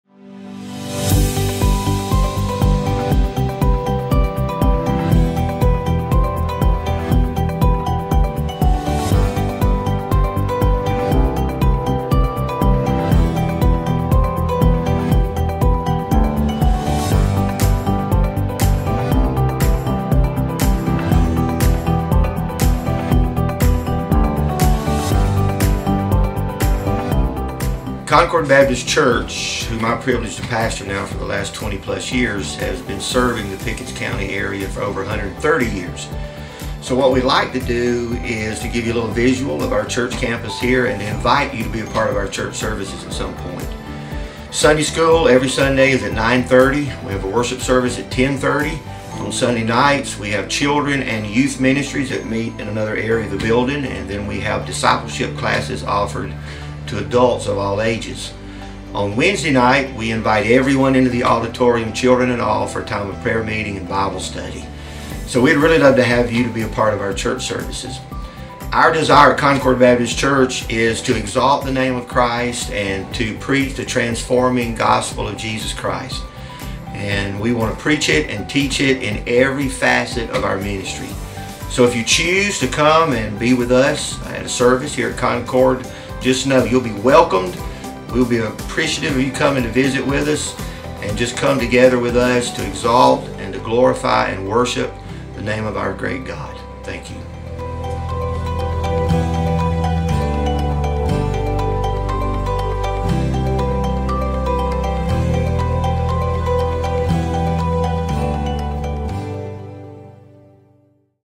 Come Join Us at CBC
Address: 723 Concord Church Rd, Pickens SC.

Concord Baptist Church wants to invite you to come see what CBC is all about.

Sunday Services:
   - Sunday School           9:30 am
   - Worship Service:     10:30 am
   - Evening Service:        5:30 pm

Wednesday Service:      7:00 pm

Video created by SC Creative Designs, LLC.